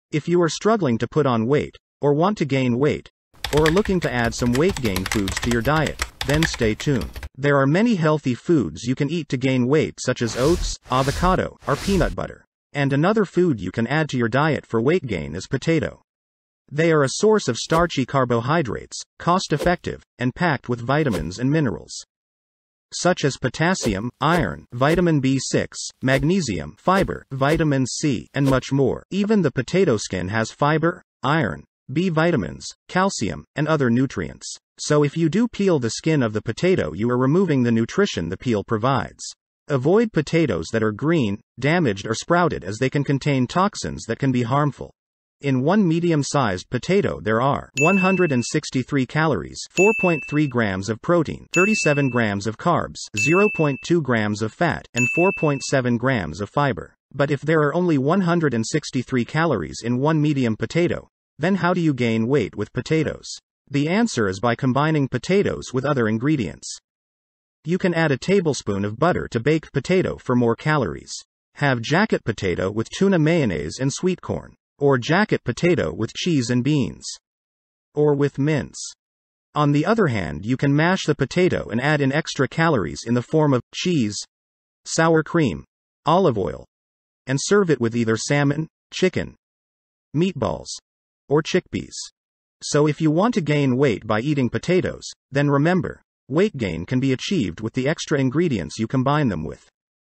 Gain Weight With Potatoes | Prime Weight Gain
Potatoes are a starchy vegetable containing many nutrients and vitamins.

Even the peel of a potato contains B vitamins, iron, and much more.

You will not gain weight by eating potatoes on their own. But you can gain weight if you combine potatoes with other ingredients.